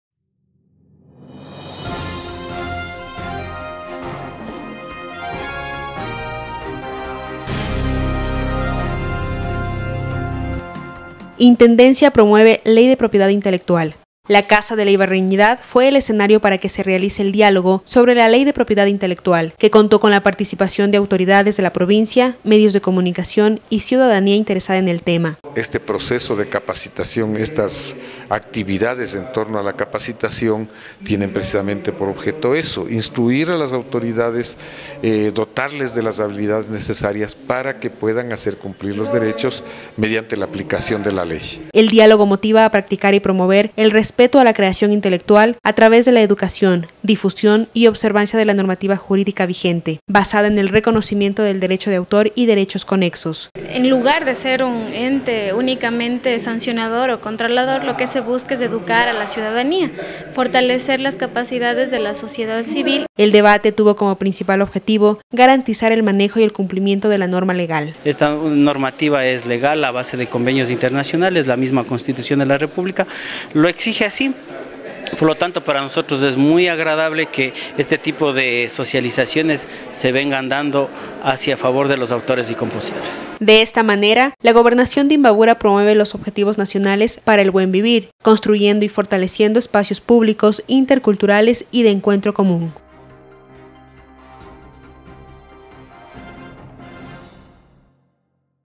Intendencia promueve Ley de Propiedad Intelectual
La Gobernación de Imbabura a través de la Intendencia General de Policía, organizaron un espacio de diálogo y debate sobre la LEY DE PROPIEDAD INTELECTUAL, cuyo objetivo fue garantizar el efectivo manejo y cumplimiento de la norma legal. De esta manera se promueve los objetivos nacionales para el Buen Vivir, construyendo y fortaleciendo espacios públicos interculturales y de encuentro común.